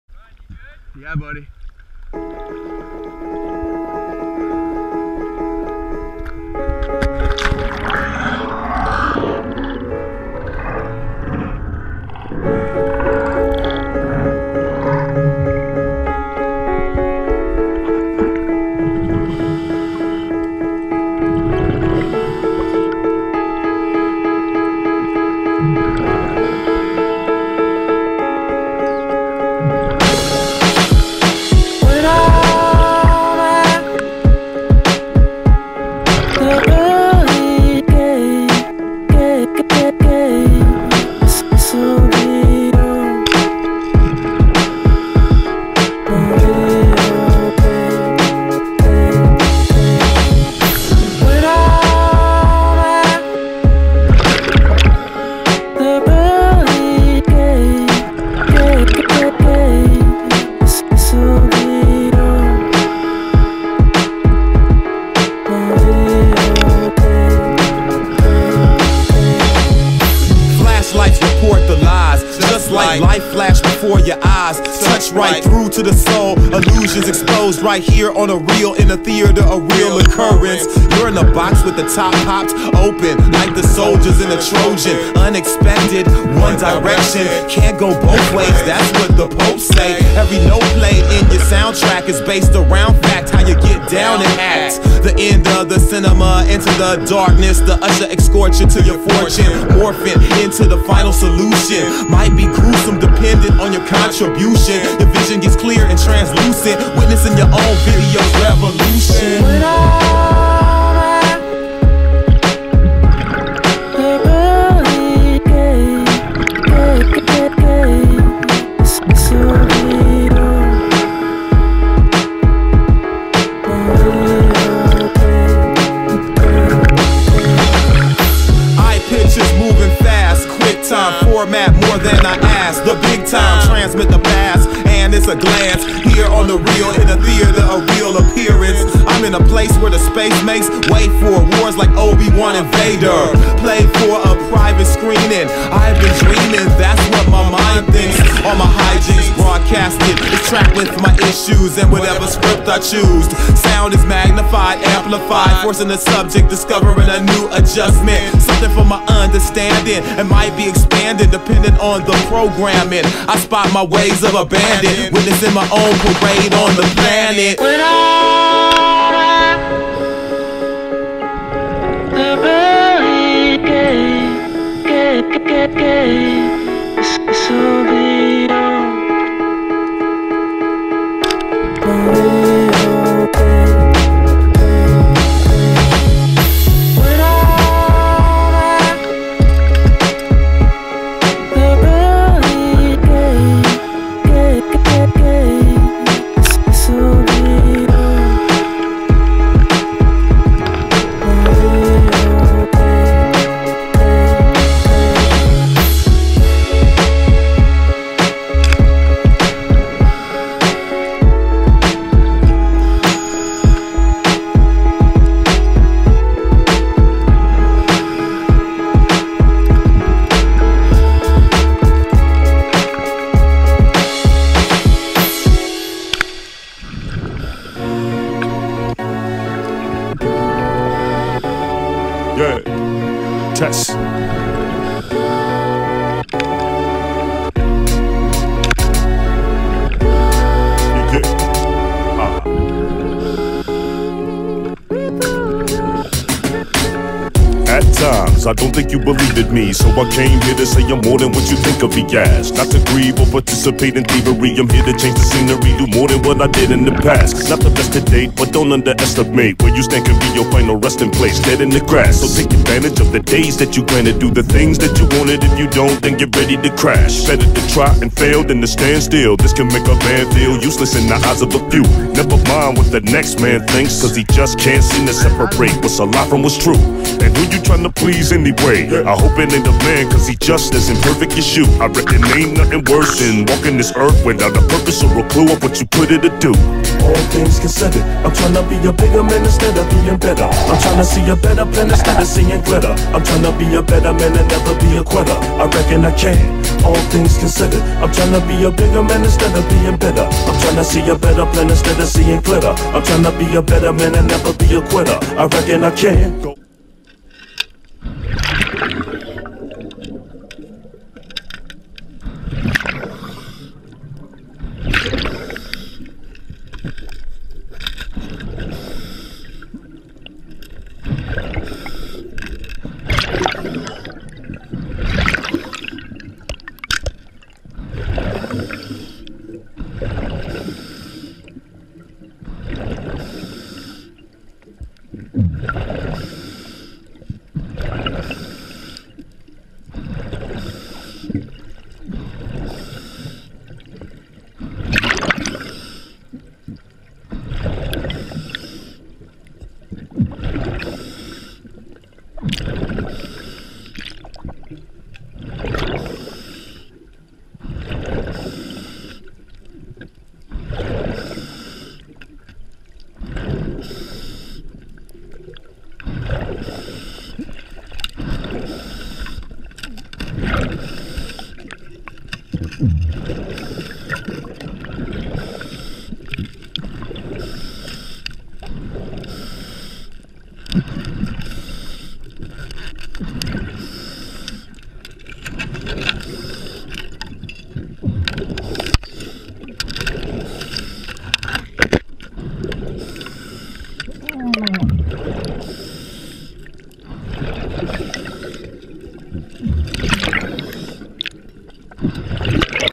Land Sick Day
Some spearing with Cousin Matt and Uncle Kent.